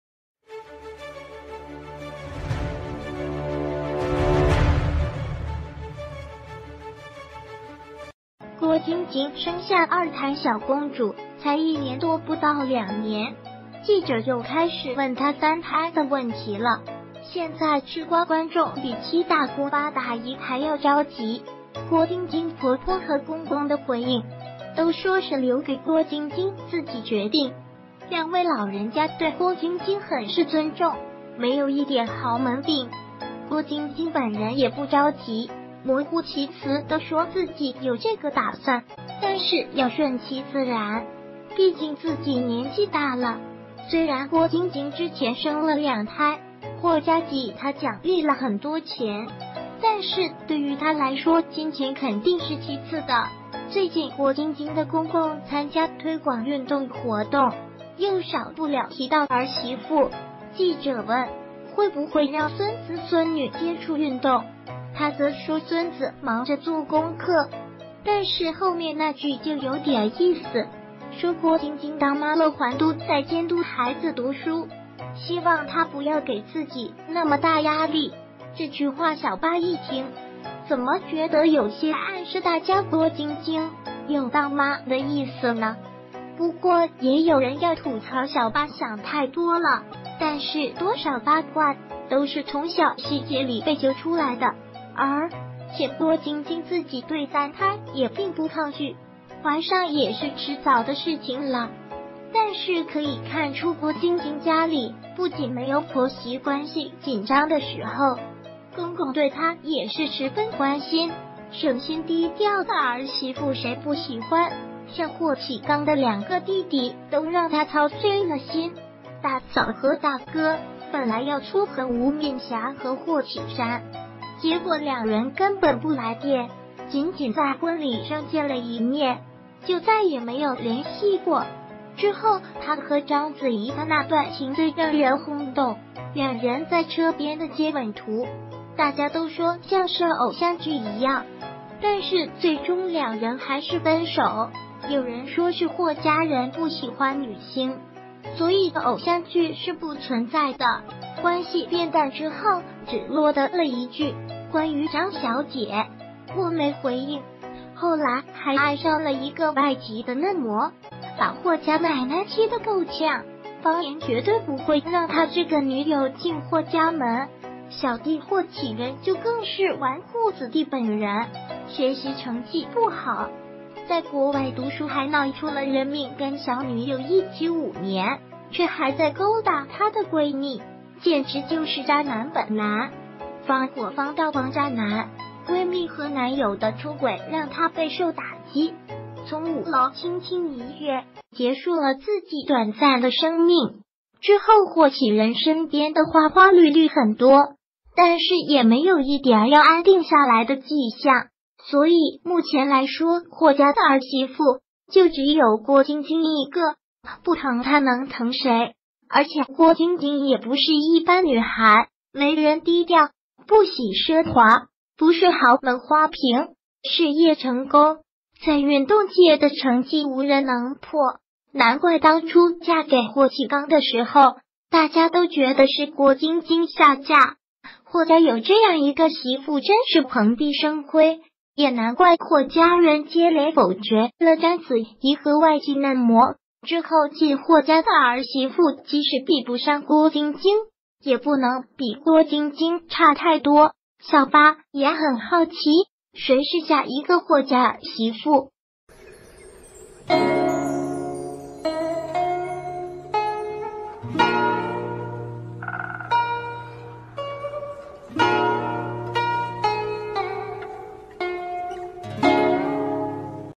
霍震霆心疼郭晶晶壓力大，身懷三胎還要教兒子讀書寫字？
郭晶晶生下二胎小公主...